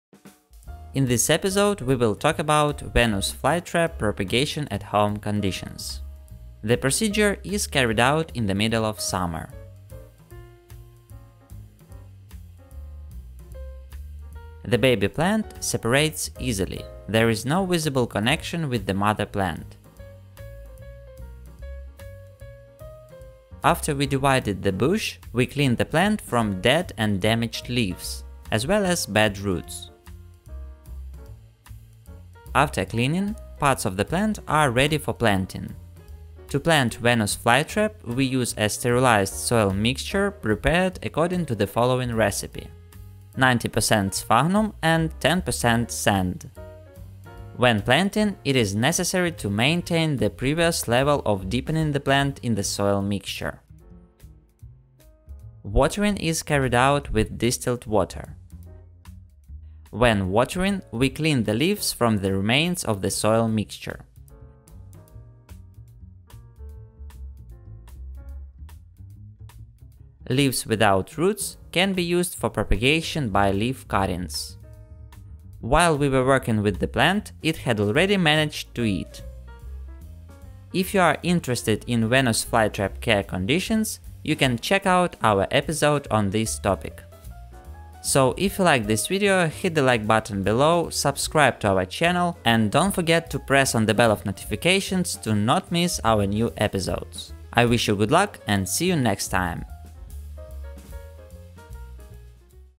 VENUS FLYTRAP PROPAGATION BY DIVIDING THE BUSH
In this episode we will talk about venus flytrap propagation at home conditions.
The procedure is carried out in the middle of summer.
The baby plant separates easily, there is no visible connection with the mother plant.
After we divided the bush, we clean the plant from dead and damaged leaves, as well as bad roots.
After cleaning, parts of the plant are ready for planting.
To plant Venus flytrap, we use a sterilized soil mixture prepared according to the following recipe:
90% sphagnum and 10% sand
When planting, it is necessary to maintain the previous level of deepening the plant in the soil mixture.
Watering is carried out with distilled water. When watering, we clean the leaves from the remains of the soil mixture.
Leaves without roots can be used for propagation by leaf cuttings.
While we were working with the plant, it had already managed to eat.
If you are interested in venus flytrap care conditions, you can check out our episode on this topic